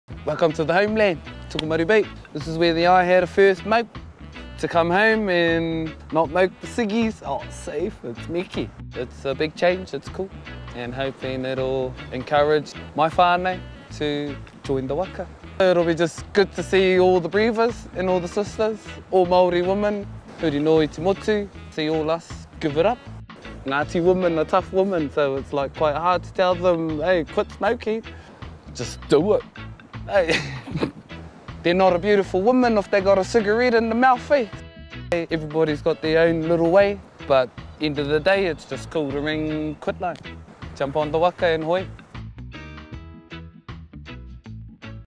Chad Chambers - Jump on the waka and become smokefree
Chad visits his hometown in Tokomaru Bay and talks about how it feels to be smokefree in a place where he had his first cigarette.